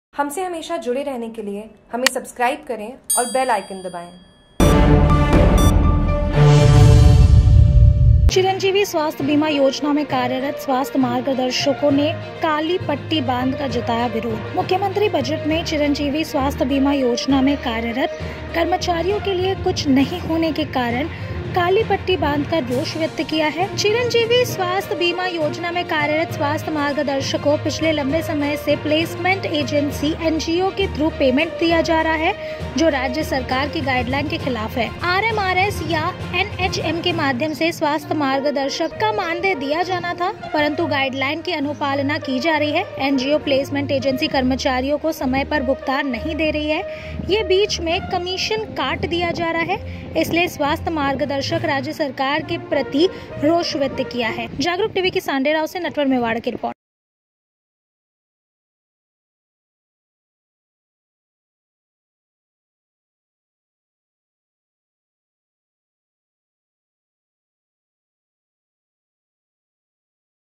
चिरंजीवी स्वास्थ्य बीमा योजना में कार्यरत स्वास्थ्य मार्गदर्शकों ने काली पट्टी बांधकर जताया विरोध
मुख्यमंत्री बजट में चिरंजीवी स्वास्थ्य बीमा योजना में कार्यरत कर्मचारियों के लिए कुछ नहीं होने के कारण काली पट्टी बांधकर रोष व्यक्त किया है। चिरंजीवी स्वास्थ्य बीमा योजना में कार्यरत स्वास्थ्य मार्गदर्शकों पिछले लंबे समय से प्लेसमेंट एजेंसी ngo  के थ्रू पेमेंट दिया जा रहा है जो राज्य सरकार की गाइडलाइन के खिलाफ है। आरएमआरएस rmrs  या nhm  के माध्यम से स्वास्थ्य मार्गदर्शक  का मानदेय दिया जाना था परंतु गाइडलाइन की अनुपालना की जा रही है। ngo प्लेसमेंट एजेंसी कर्मचारियों को समय पर भुगतान नहीं दे रही है यह बीच में कमीशन काट  दिया जा रहा है इसलिए स्वास्थ्य मार्गदर्शक राज्य सरकार की प्रति रोष व्यक्त किया है।